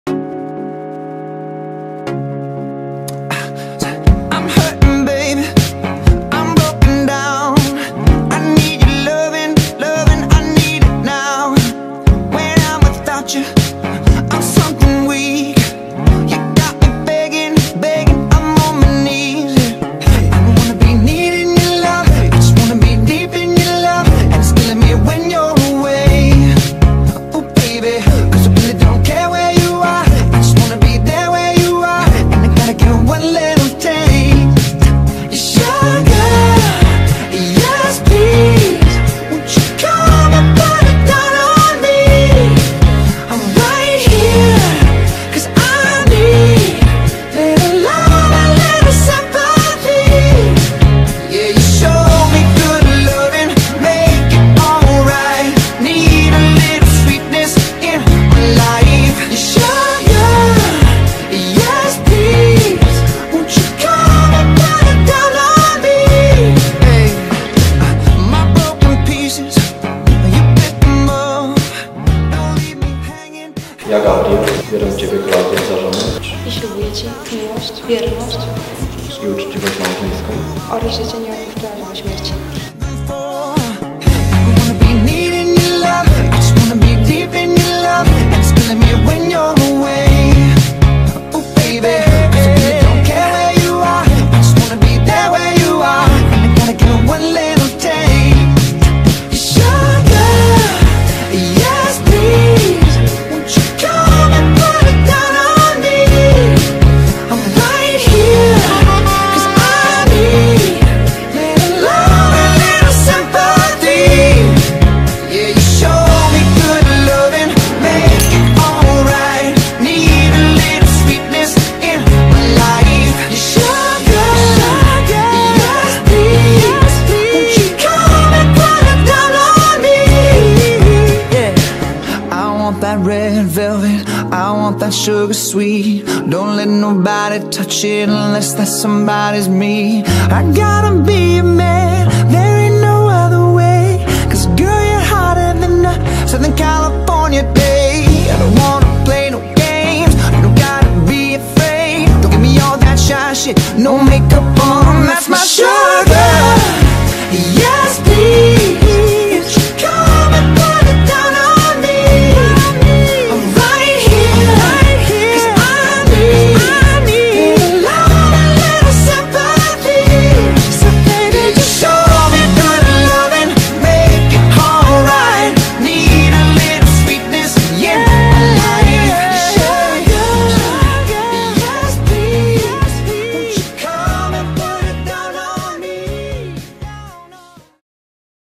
KLAUDIA I GABRIEL TELEDYSK ŚLUBNY
Krótki film z wesela Klaudii i Gabriela. Zapraszamy do polubienia naszego fanpage'a na facebooku oraz zapraszamy na stronę